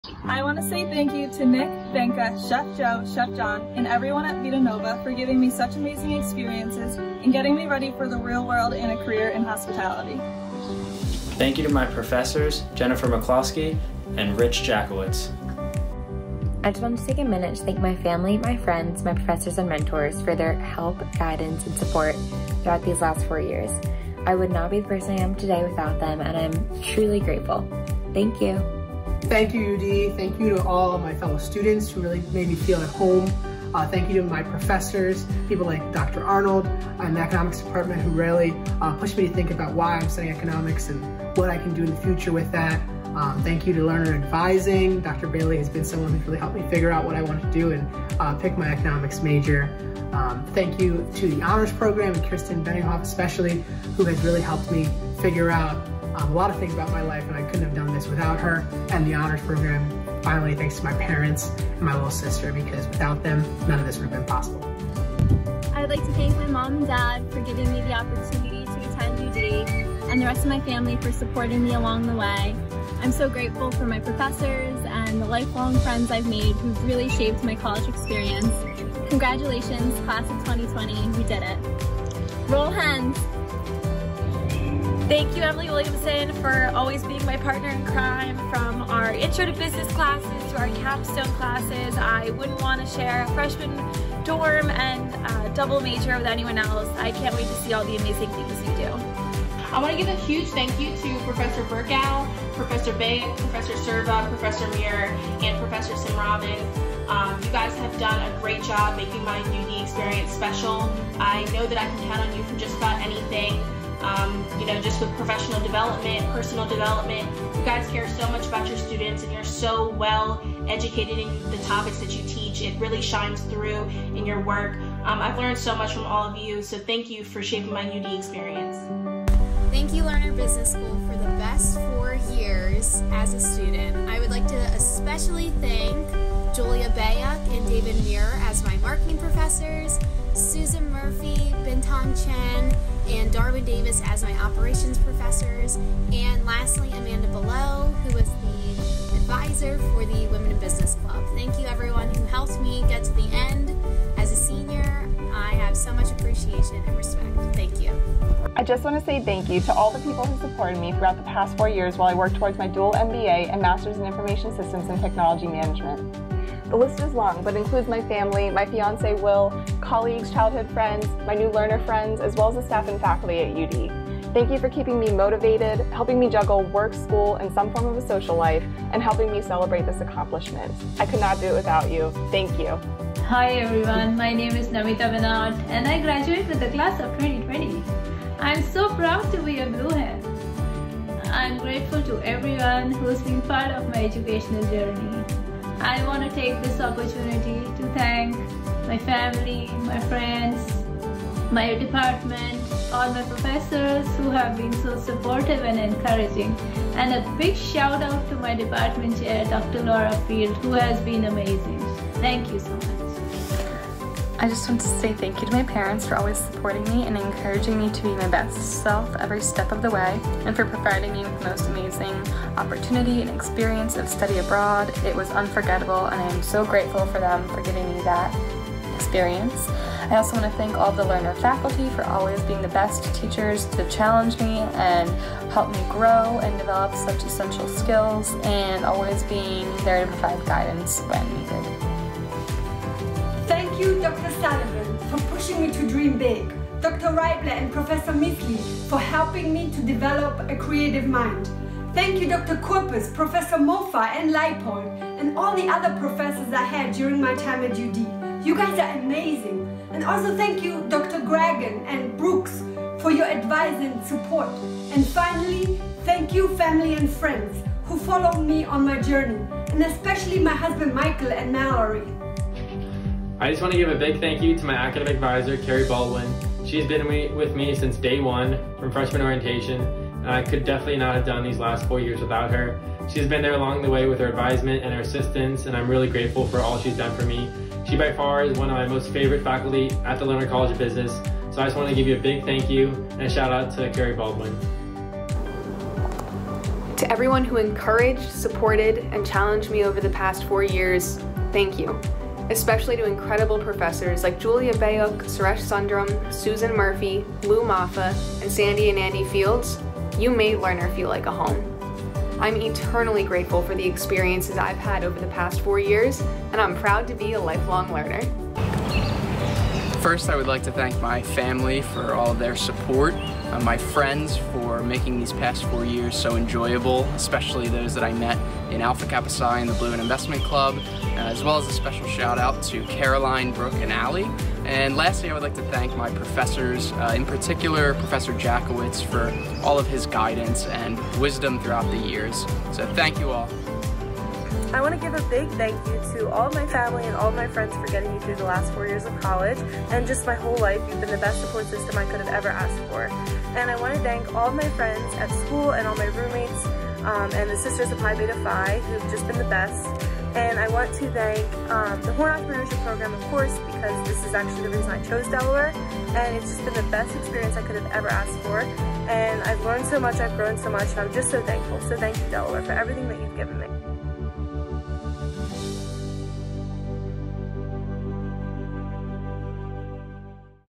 Congratulations to the University of Delaware's Lerner College Class of 2020!
Congratulations to the University of Delaware's Lerner College Class of 2020! In this video, 16 graduating students share who they want to thank as they graduate. The Lerner College is so proud of each of these Lifelong Lerners!